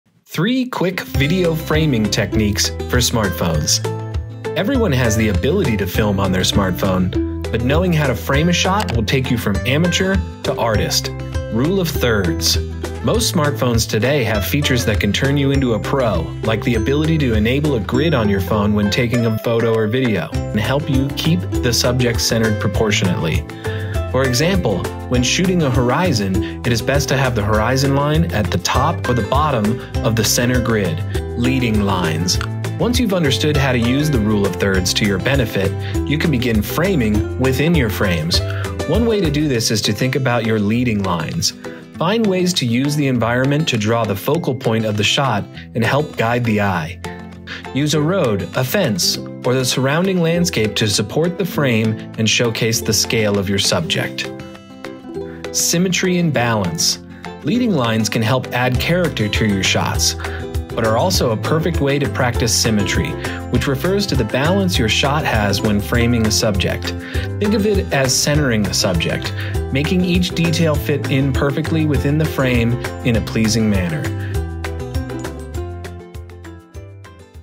3 Quick Video Framing Techniques for Smartphones
Use these steps to learn how to frame video with any smartphone. Learn rule of thirds, leading lines, and symmetry/balance.

Chapters:
00:11 Rules of Thirds
00:30 Leading Lines
00:55 Symmetry & Balance